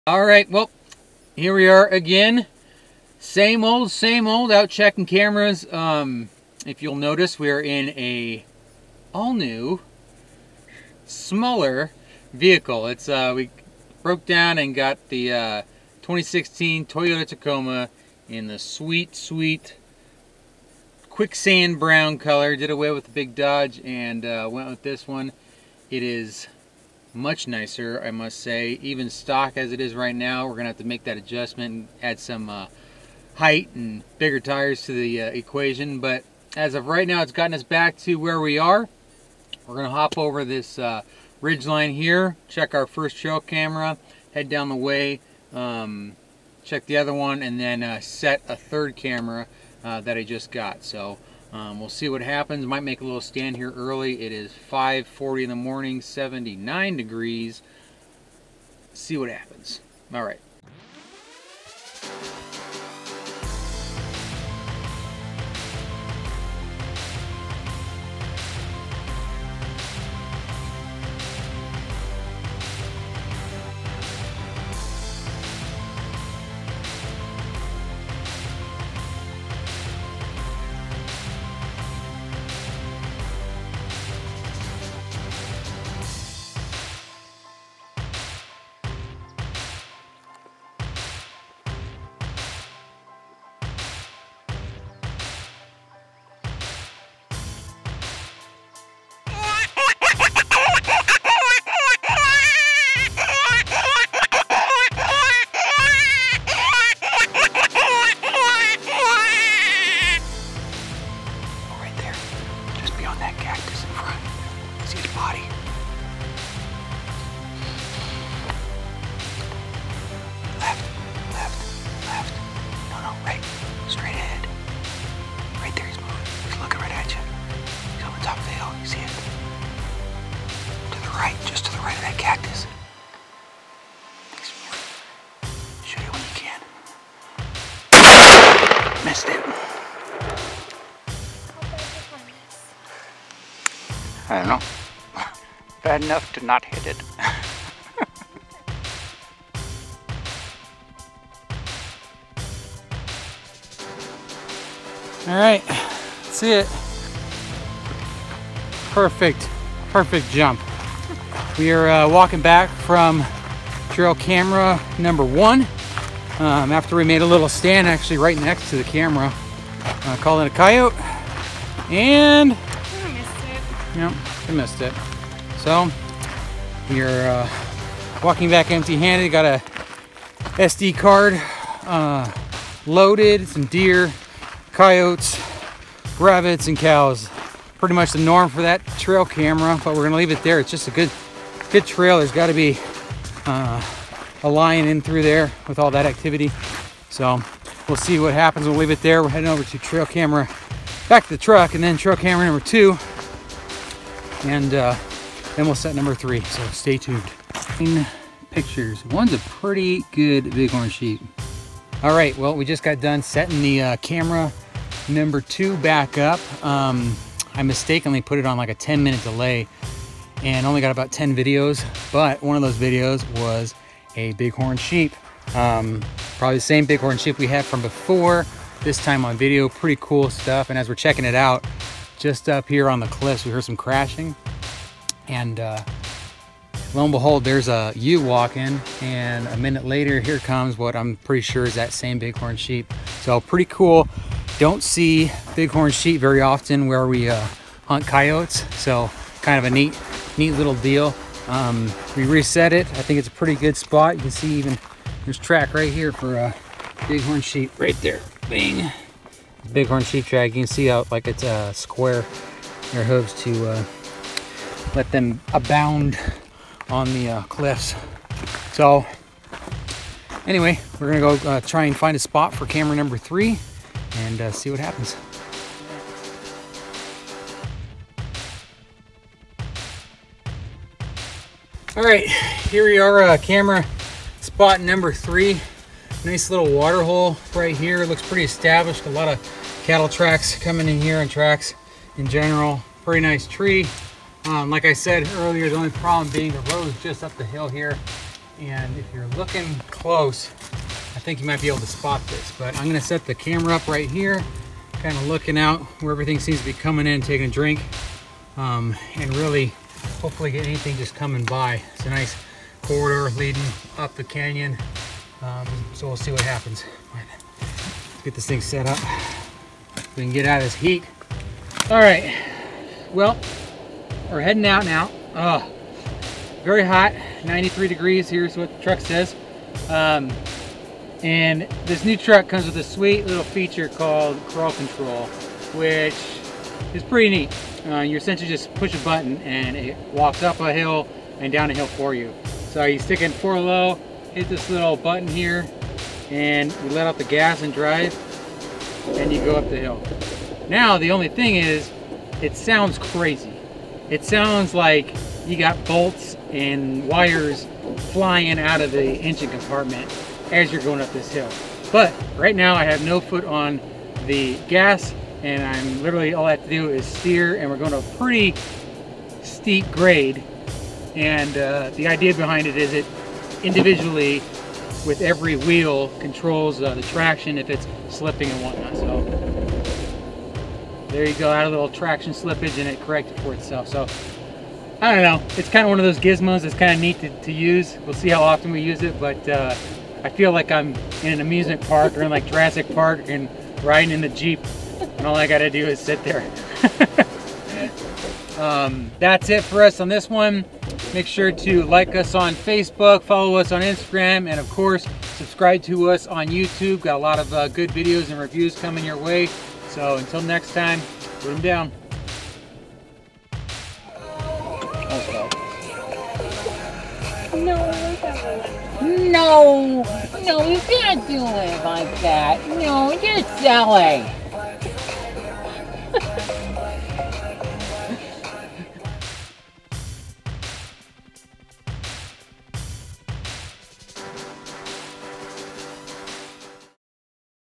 Mountain Lion Search 2016 E.9
Ross and Nicole decide to make a stand and call in a summer coyote.